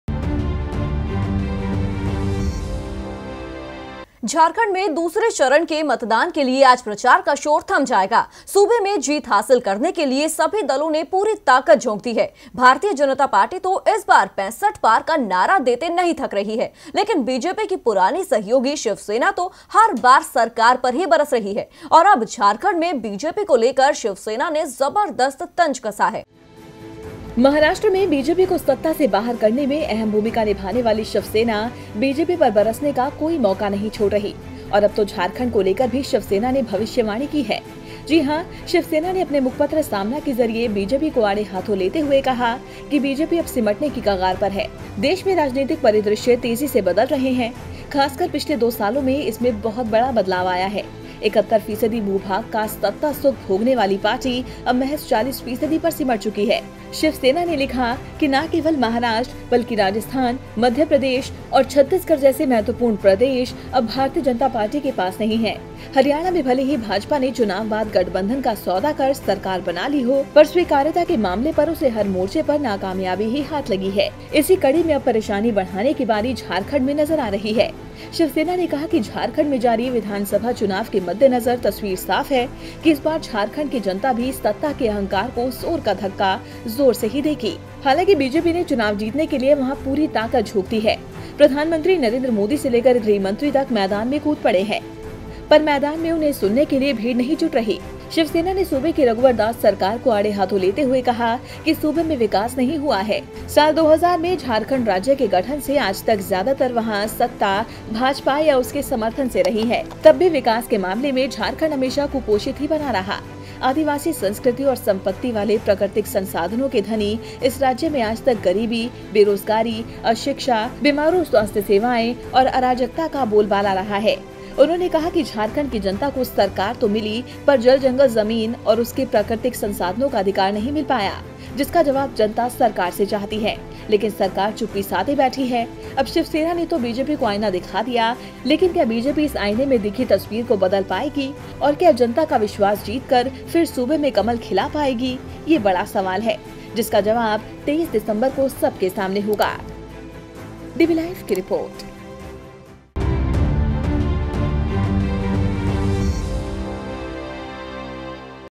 Jharkhand में BJP को मिलेगी शिकस्त ! Shivsena ने सामना में BJP को दिखाया आईना
झारखंड में दूसरे चरण के मतदान के लिए आज प्रचार का शोर थम जाएगा. सूबे में जीत हासिल करने के लिए सभी दलों ने पूरी ताकत झोंक दी है...भारतीय जनता पार्टी तो इस बार 65 पार का नारा देते नहीं थक रही है,,,,लेकिन बीजेपी की पुरानी सहयोगी शिवसेना तो हर बार सरकार पर ही बरस रही है. और अब झारखंड में बीजेपी को लेकर शिवसेना ने जबरदस्त तंज कसा है.